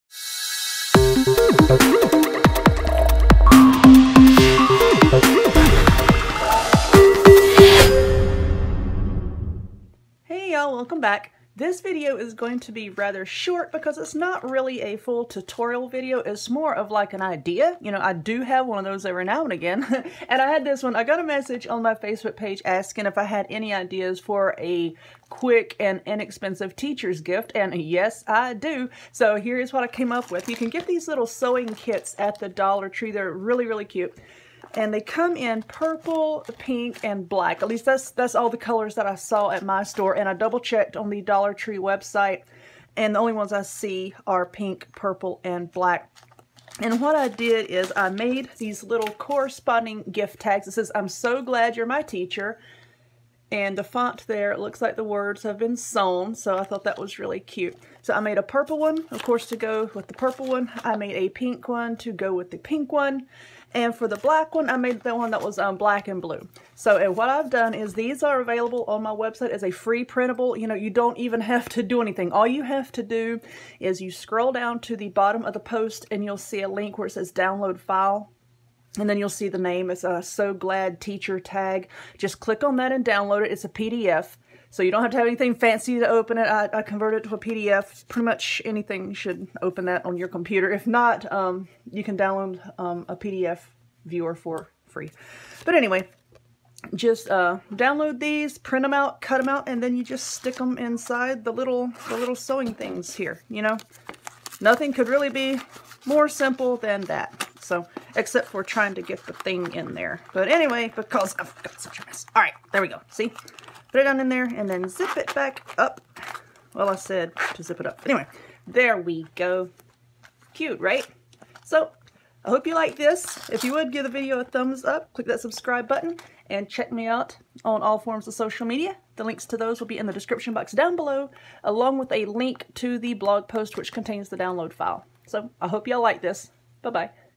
Everyday Crafting: Dollar Tree Teacher Gift with Free Printable
Hey y'all! This video is a request from Facebook for a cheap teacher's gift. This one uses a Dollar Tree sewing kit and the free printable that you can download from my

Send me some Friend Mail!

I love to hear from my friends! I will always send you some Friend Mail in return!